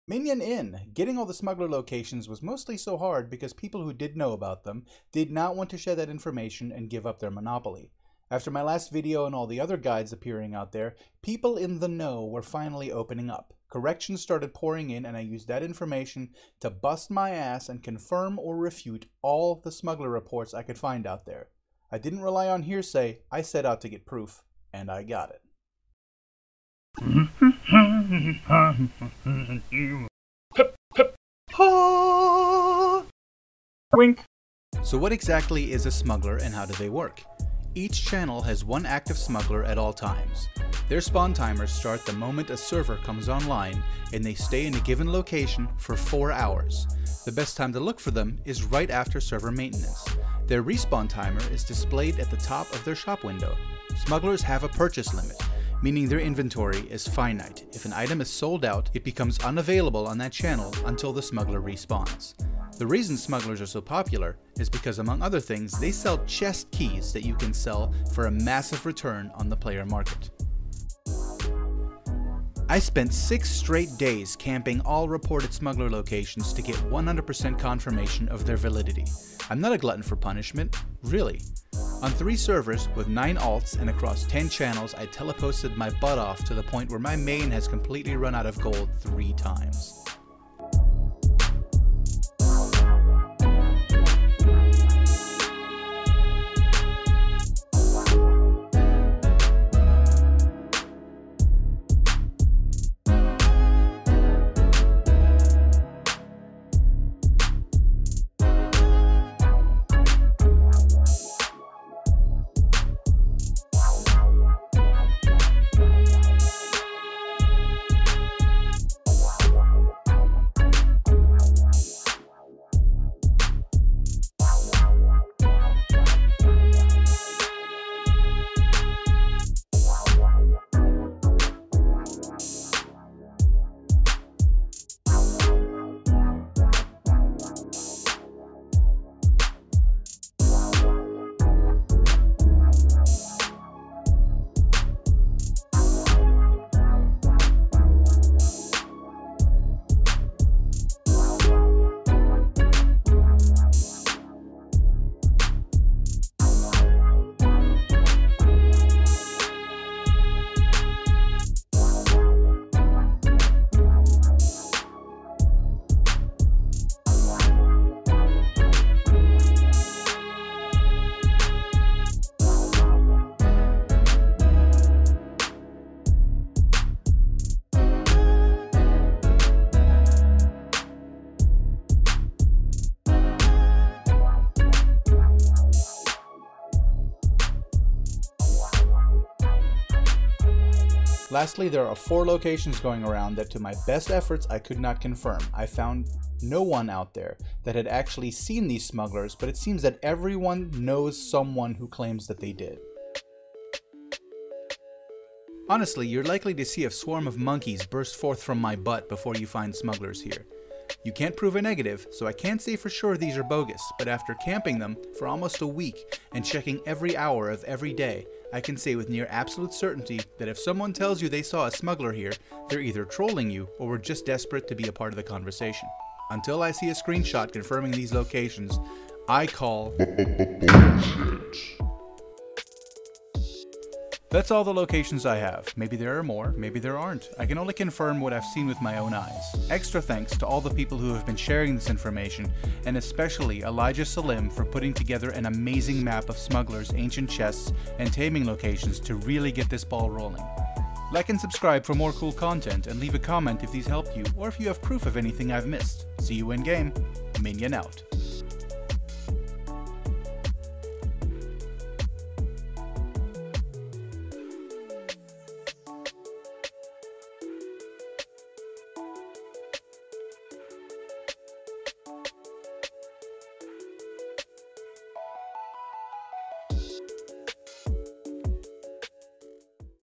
The Most Confirmed Smuggler Location Guide in the History of Ever
16 Smuggler locations and clips to prove it

Big thanks to DreamyGamerGrl for putting in the hard work to gather most of this information. Without her efforts to work from, I wouldn't have been able to confirm and debunk as much as I did.